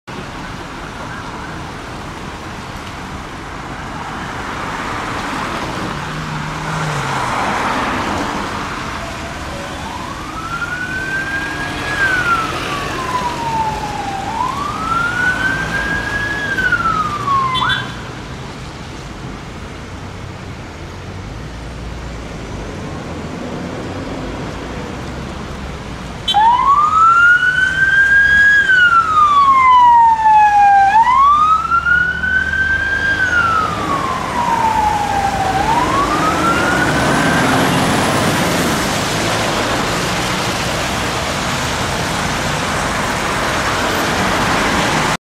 Kent Police Van Driver Training Unit on a blue light run in Maidstone!
Kent Police Vauxhall Vivaro Driver Training Unit (DTU) on a blue light training run in Maidstone, Kent.